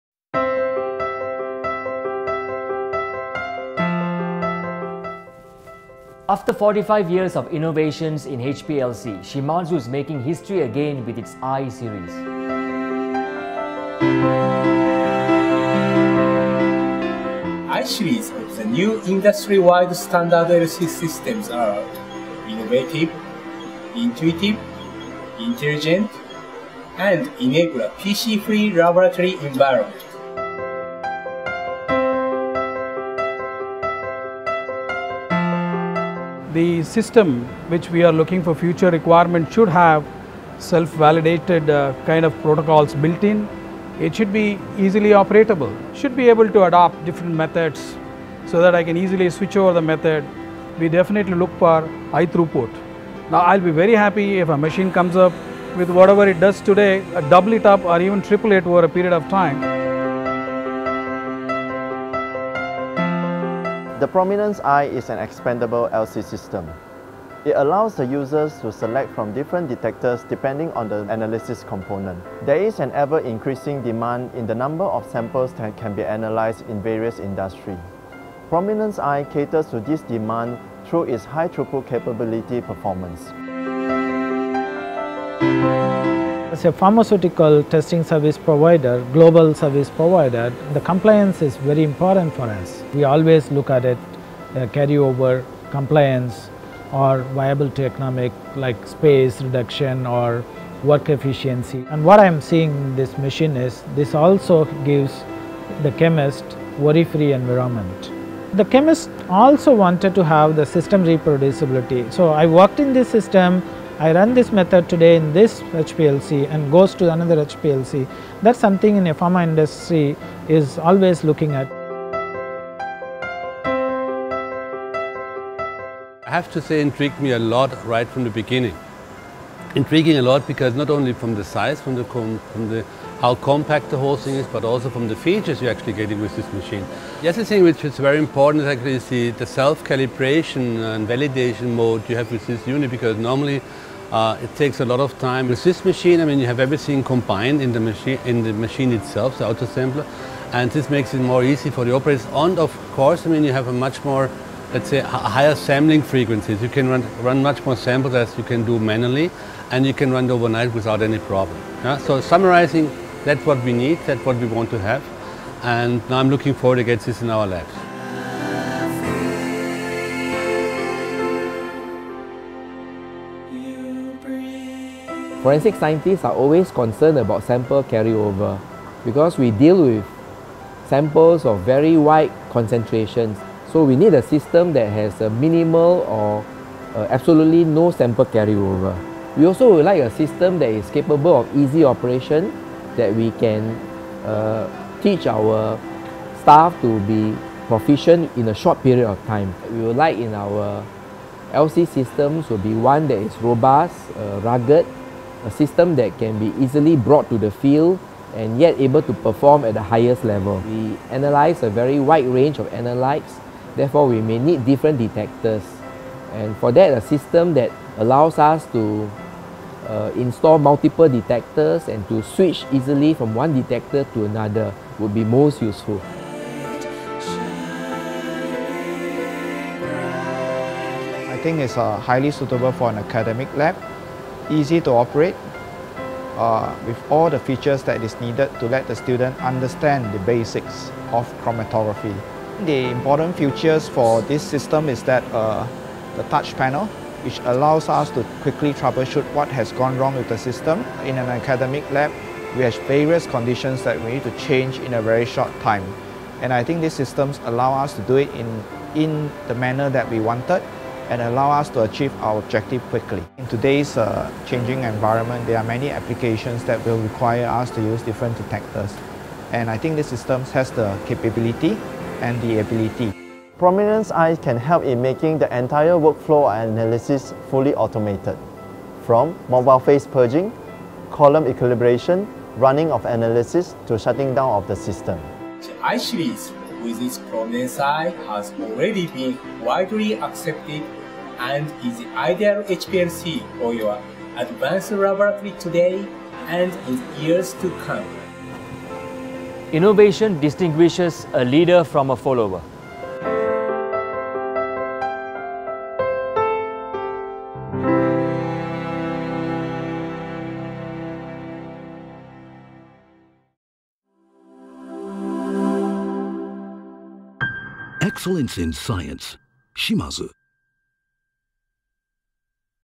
i-Series the new generation of i-volution in HPLC Analysis
First HPLC i-Series customer reviews.

The i-Series, designed for covering conventional to ultra-fast LC applications, consists of the compact HPLC Prominence-i and UHPLC system Nexera-i. High speed analysis, simplified method transfer, minimized environmental impact and easy maintenance are just a few of the many new features of this product range